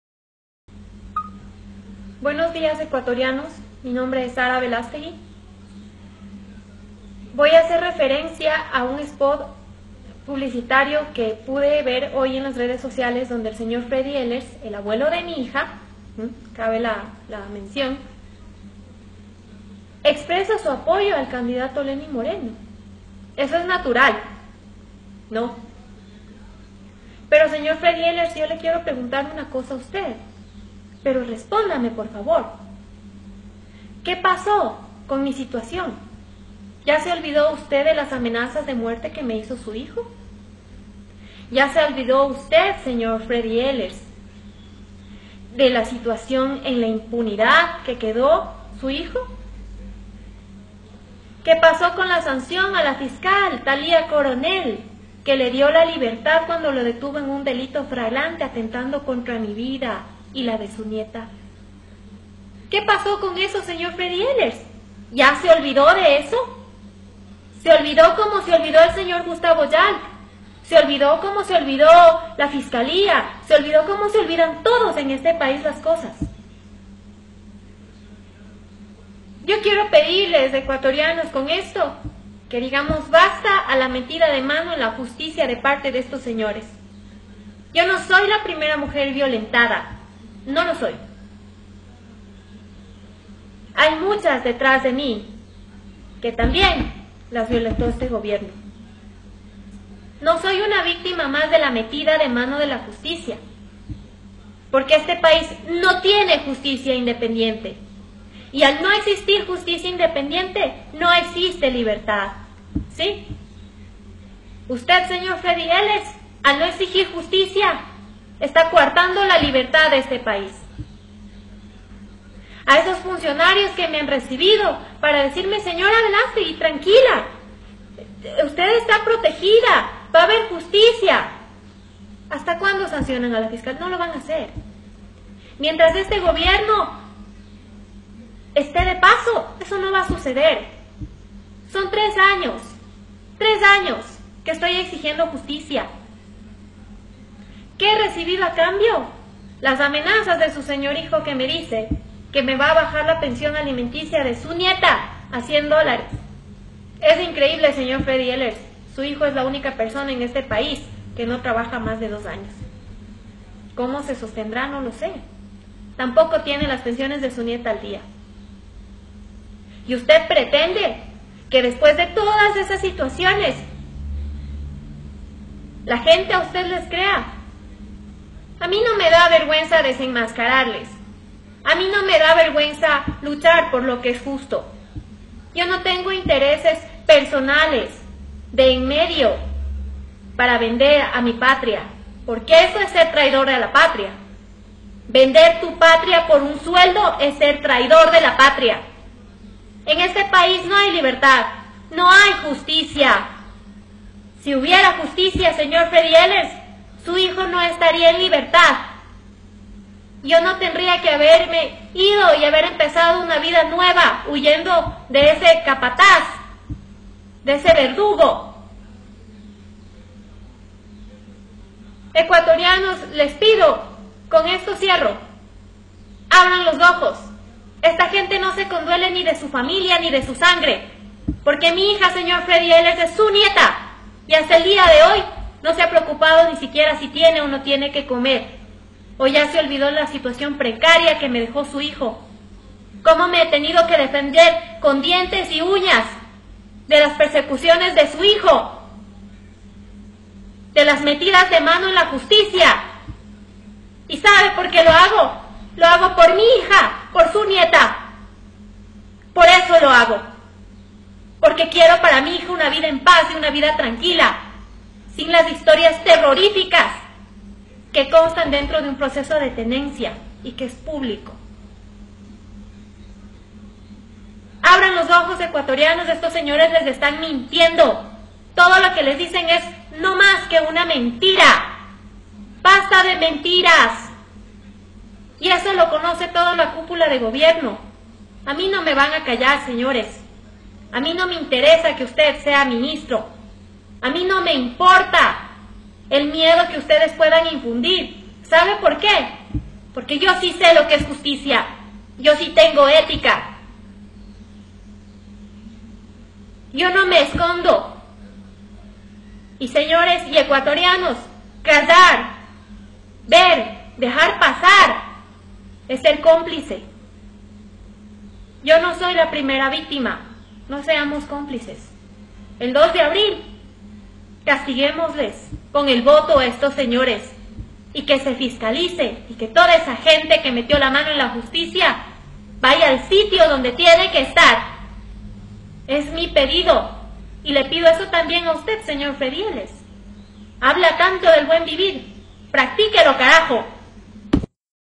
Freddy Ehlers es acusado por su Ex Nuera Sara Velastegui
Es increíble que por la ambición del dinero y del poder las personas pierdan el horizonte y mucho menos la dignidad de mantener a la familia unidad